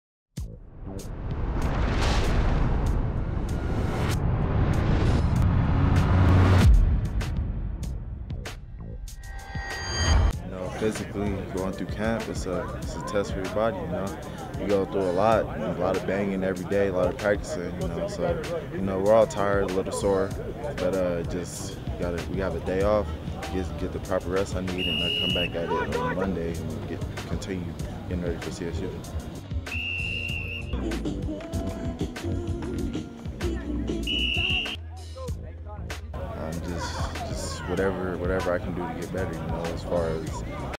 2013 Fall Football Camp Day 18
featuring an interview with Christian Powell